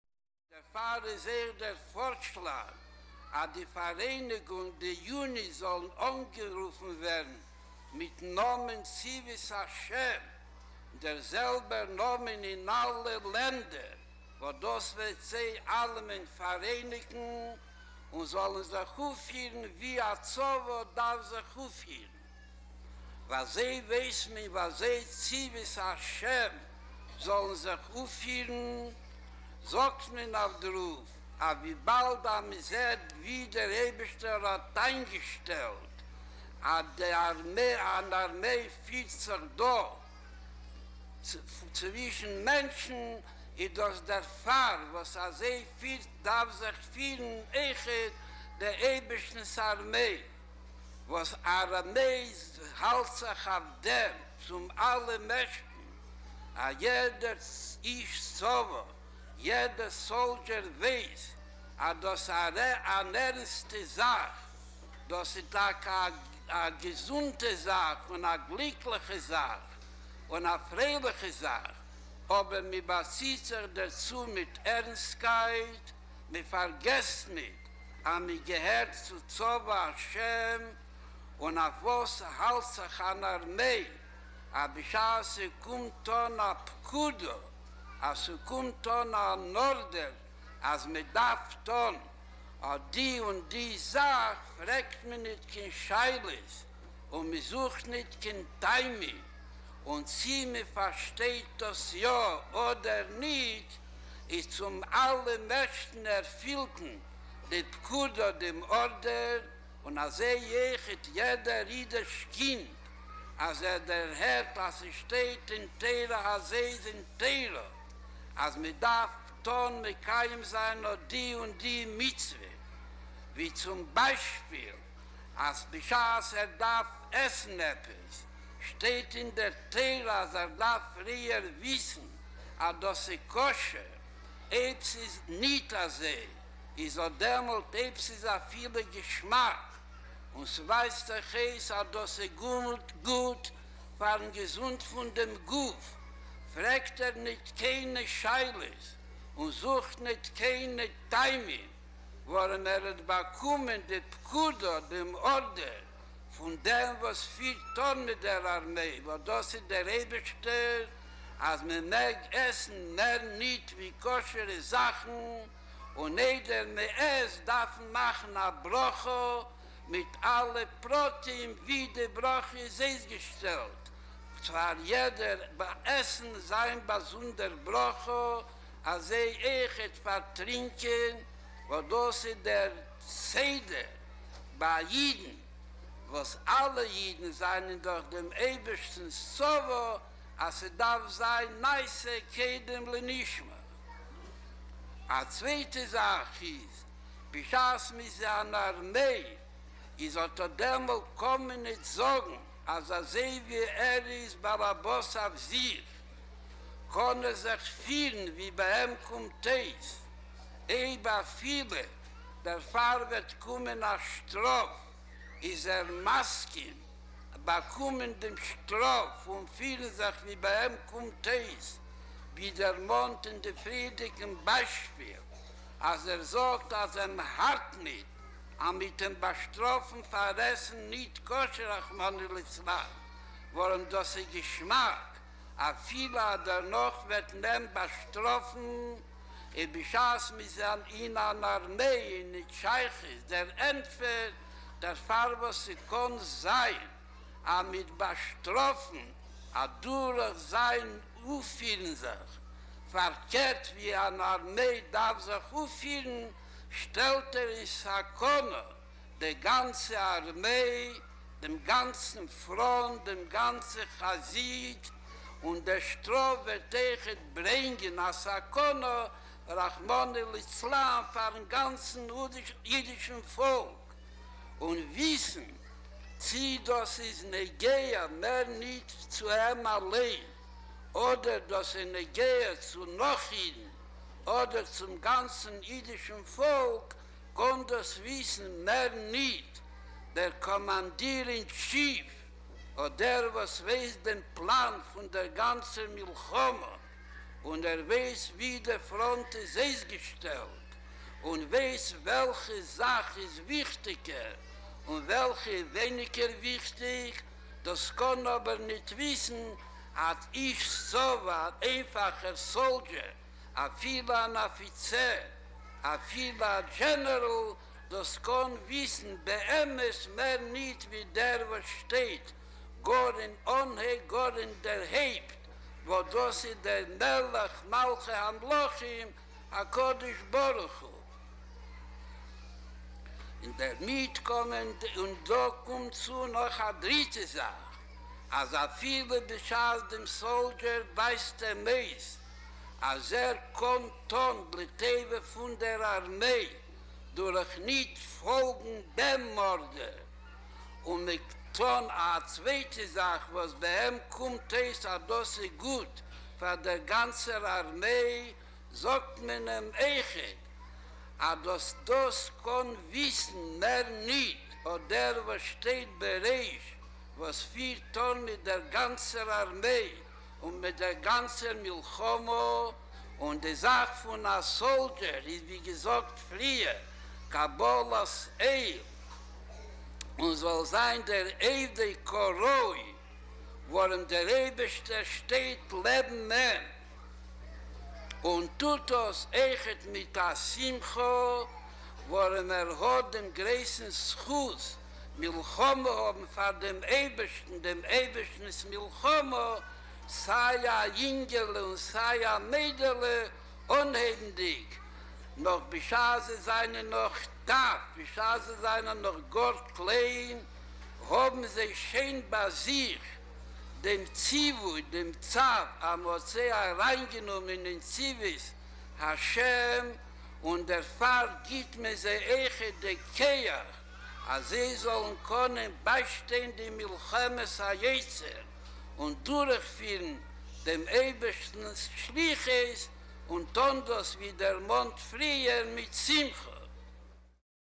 Tzivos Hashem – G-d’s Army – An Inaugural Speech
This Sukkos marks the 40th anniversary of the founding of Tzivos Hashem!

Addressing a special rally for children held by the Rebbe in 770 on the 19th of Tishrei, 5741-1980, the Rebbe announced the formation of a new club for Jewish children worldwide called "Tzivos Hashem".

Complete with ranks, missions, and regalia, Tzivos Hashem, would provide a unique educational tool for instilling Jewish pride and values as well as a strengthened sense of identity to generations of Jewish youth.

This historic event, recorded in its entirety by the JEM film crew, has been remastered for optimum viewing and is available now

Living Torah Disc 15 Program 58